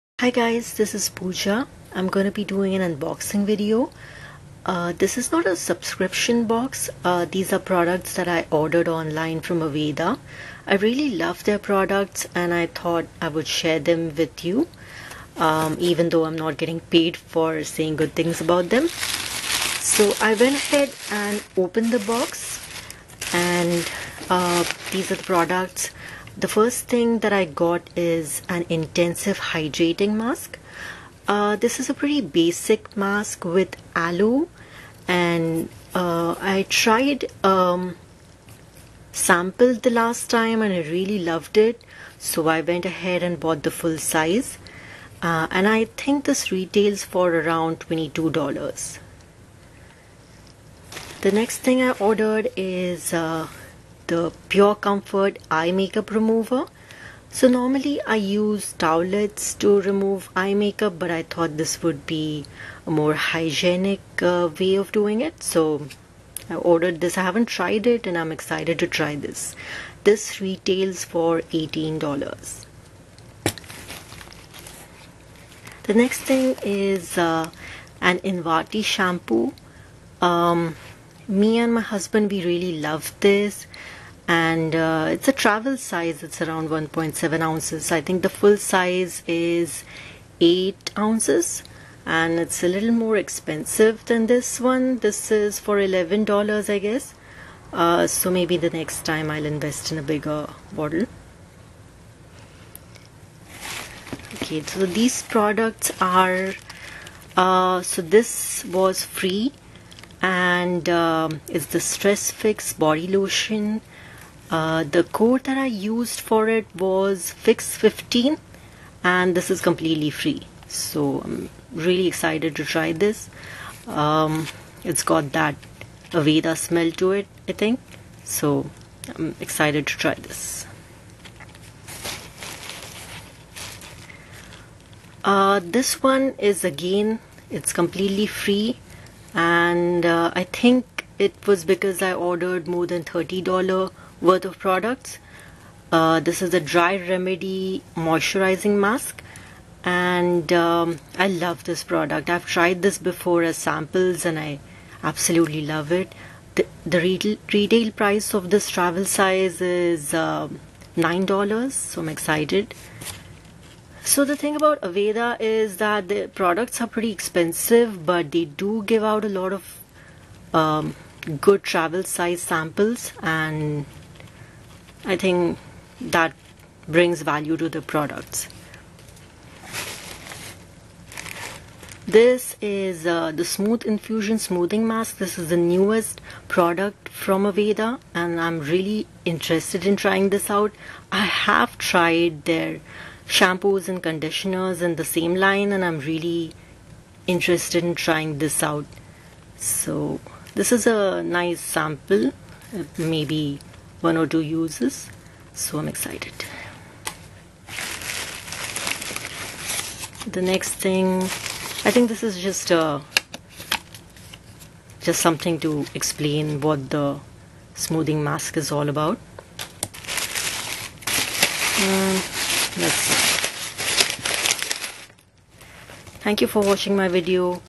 First unboxing!!! Aveda Haul!
intensive hydrating masque $22
pure comfort™ eye makeup remover $18
invati™ exfoliating shampoo $11

Free Samples with this order:
free travel size dry remedy™ moisturizing masque
free sample of smooth infusion™ smoothing masque